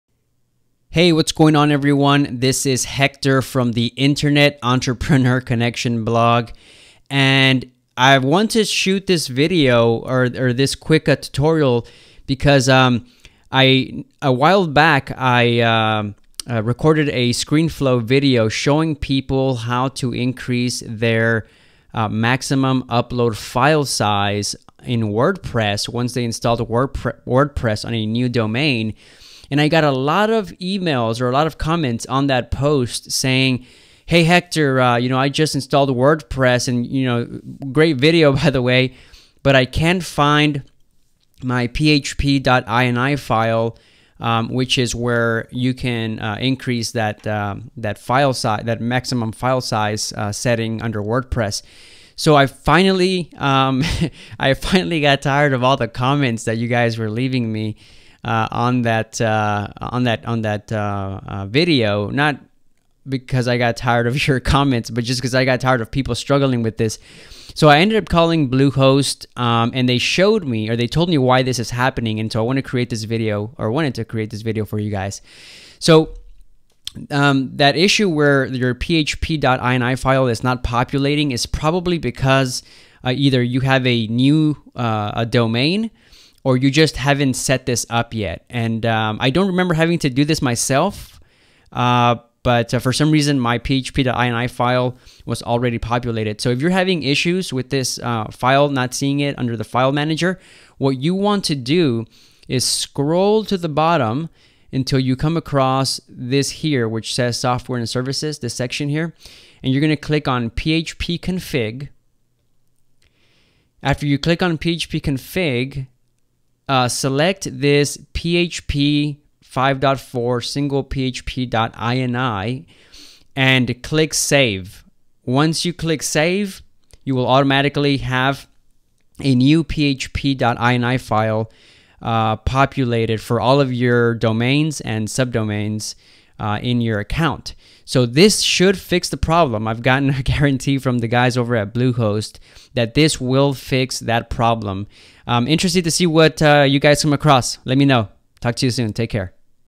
How to create your "php.ini" file in Bluehost if it is Missing in your "File Manager"
Okay, so a few months back I created a video where I showed you "How to increase the maximum upload file size" in Wordpress.

This required you to sign-in to your Bluehost.com CPanel and edit the "PHP.INI" file from your "File Manager" / "Public_html".

HOWEVER, it seems like a lot of people simply did not have the "php.ini" file populated/enabled in their file manager and therefore were not able to edit this file to increase the maximum upload file size in Wordpress.

I called Bluehost to settle this once and for all and this is what they guaranteed me would fix the problem.

I hope this helps you guys! Let me know if anyone else runs into other issues doing this.